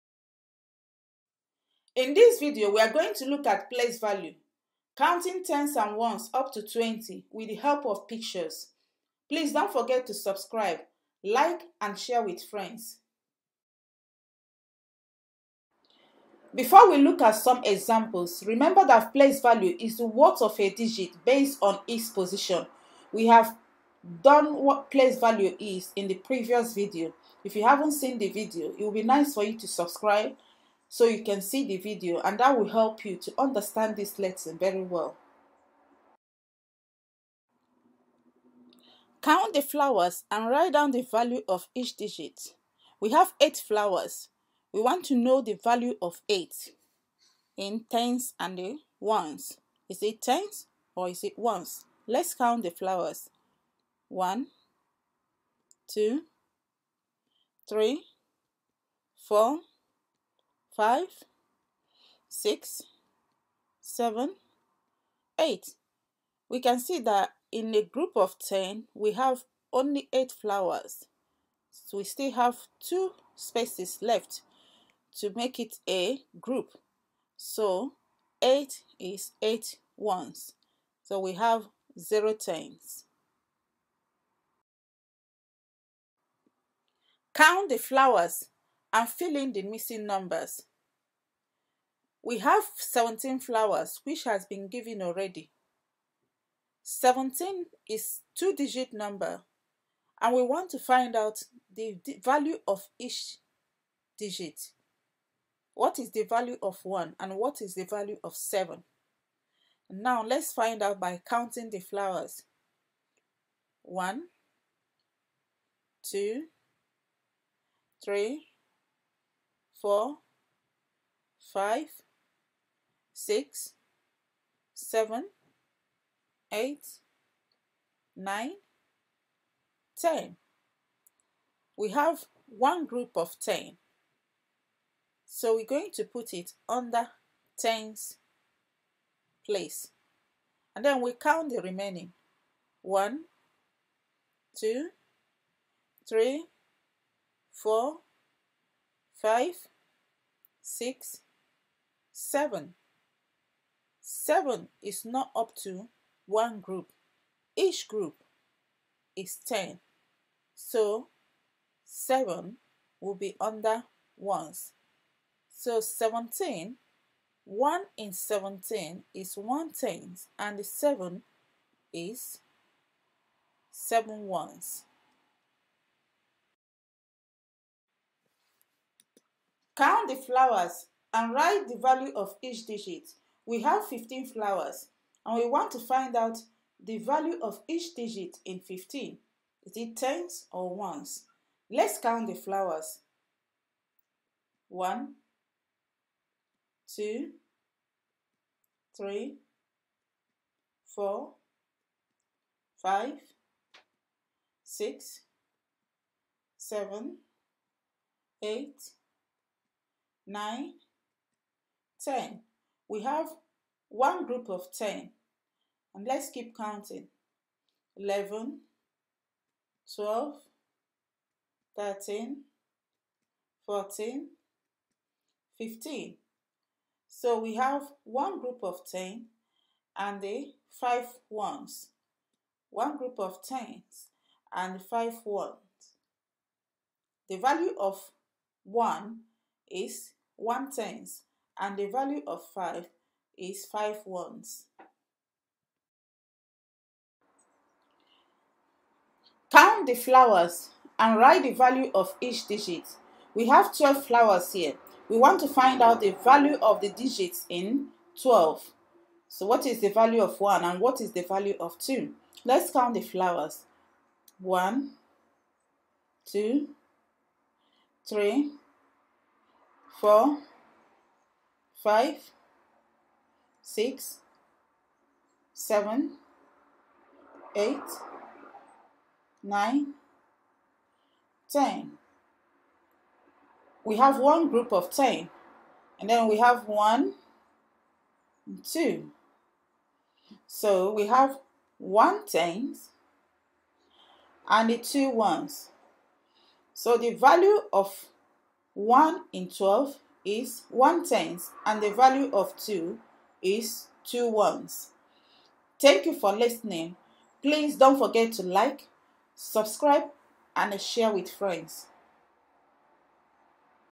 Place Value || Counting Tens and Ones Up to 20 || Mathematics made easy for kids.
Place Value Counting Tens and Ones Up to 20